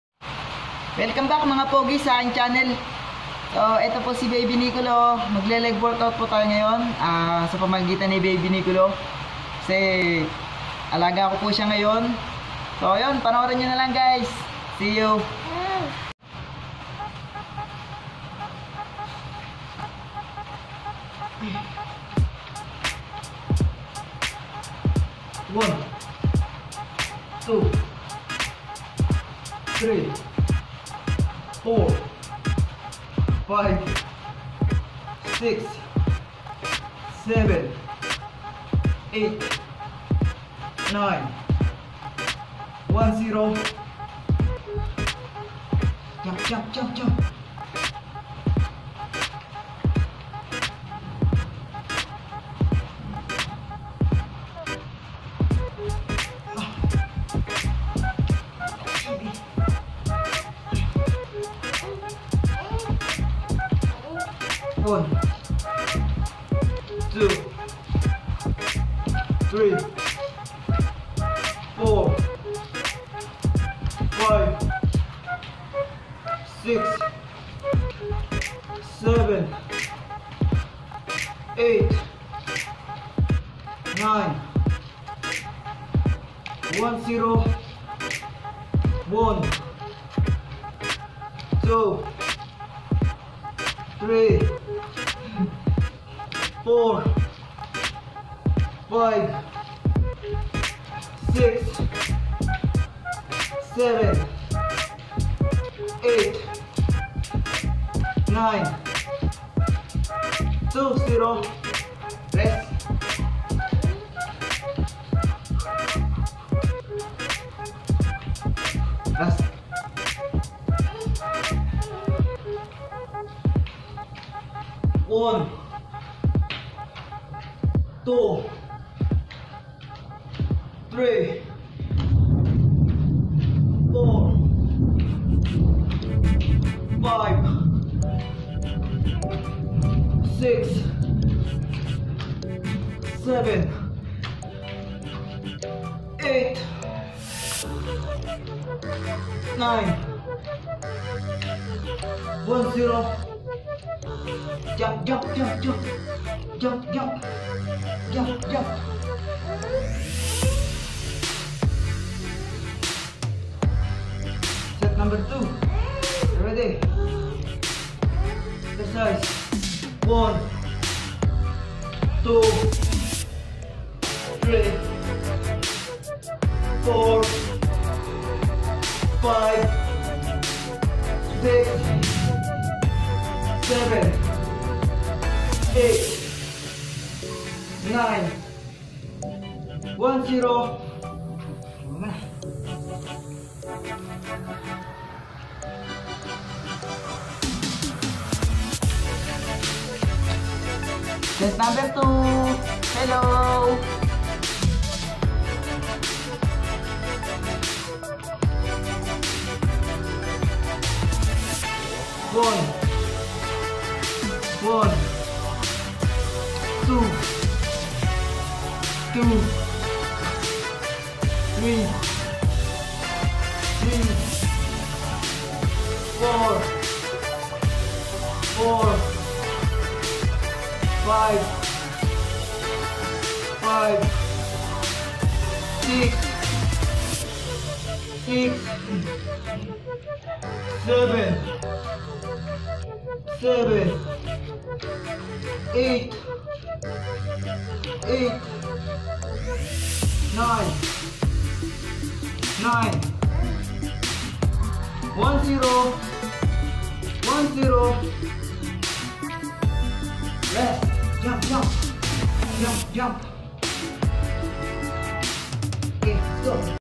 Leg WorkOut With Baby Nicholo💪
Dahil Fathers Day kahapon,. Alagaan ko muna ang Baby. Habang nag exercise. 😂😊
Pwede din ninyong subukan ito. Effective at talagang mananakit ang mga legs niyo.

Tandaan depende pa din sa bigat ng mga alaga ninyo ang inyong bubuhatin para maiwasan ang injury.

So yun lang next na exercise natin ay mas marami pang reps..

Magcomment ka sa baba. Para maishot out kita sa mga gagawin ko pang videos..

God bless